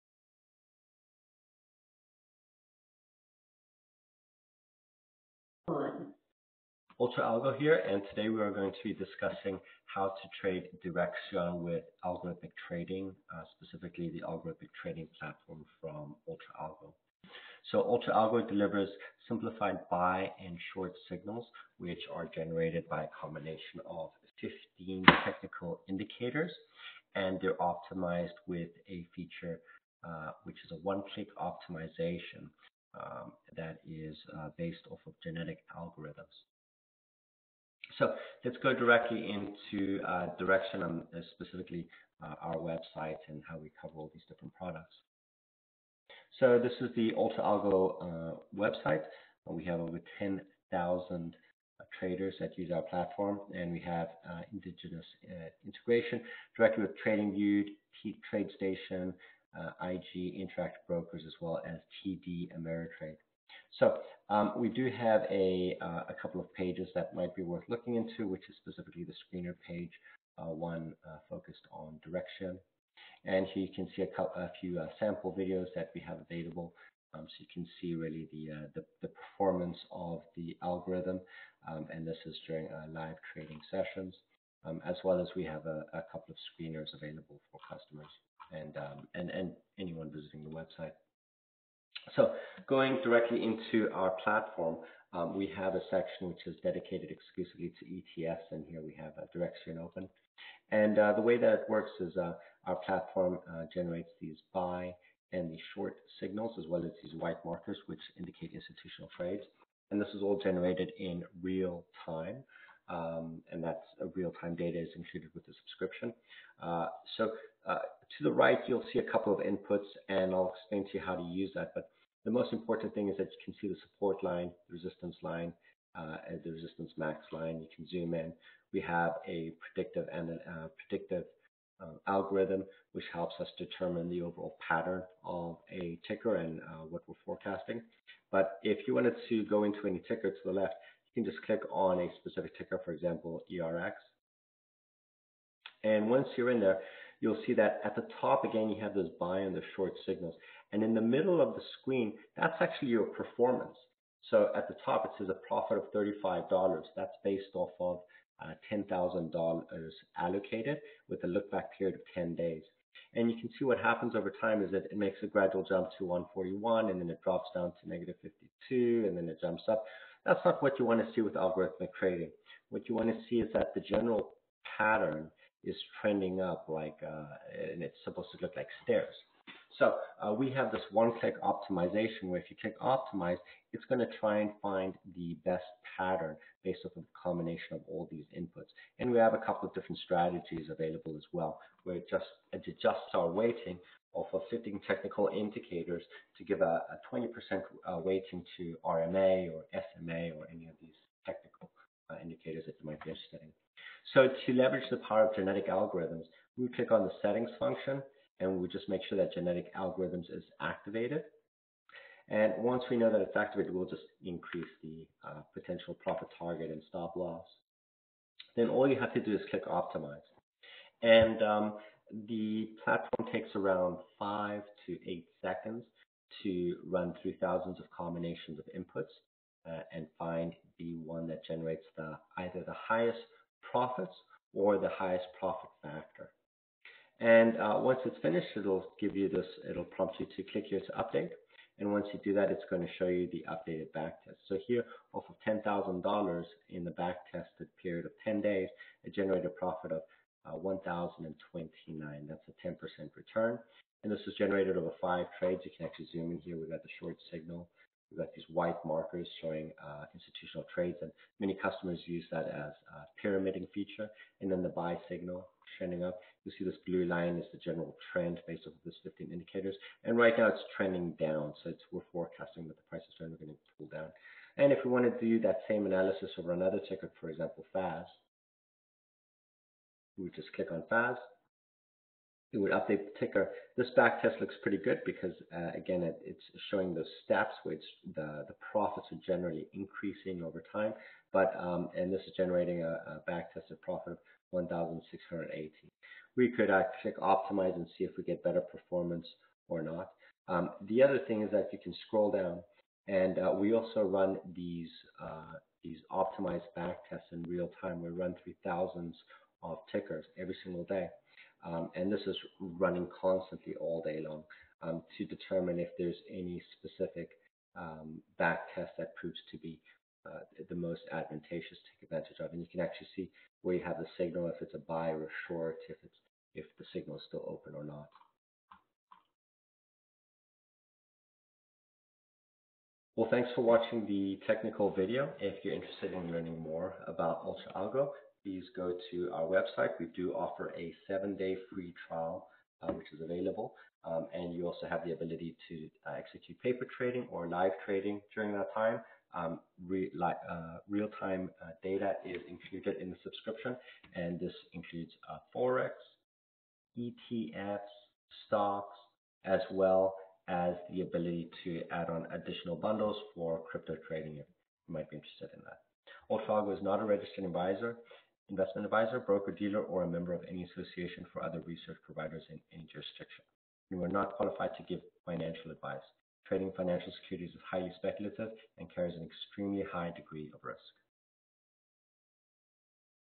Direxion ETFs | Algorithmic Trading Leveraged & Inverse ETF
Direxion provides both Leveraged & Inverse ETFs across multiple market sectors, including: Commodities, Emerging Markets, Finance, Healthcare, Technology and many more. Leveraged products include 1X, 2X, 3X which relates to the target daily investment results before fees and expenses.

Algorithmic trading across some of the best ETFs involve identifying patterns that deliver consistent returns based on multiple performance metrics. Trading across sectors allows investors to manage risk by reducing exposure to any single security. However, leverage products do carry a higher range of risk during times of volatility. It's important to consider the fees associated to holding leveraged products when calculating returns.

To generate consistent performance, we utilize backtesting across Profit Factor (Gross Wins divided by Gross Losses) and Profitability (Percentage of Winning Trades relative to Total Trades executed). Most technical traders are less concerned on the overall fundamentals relating to the security. Volatility is a major consideration when reviewing performance, specifically as it relates volume and potential indications of institutional support in either direction. Though ETFs cannot be directly manipulated since they comprise of a basket of securities, our institutional markers can pin-point general inflows and outflows of funds across the market as a whole.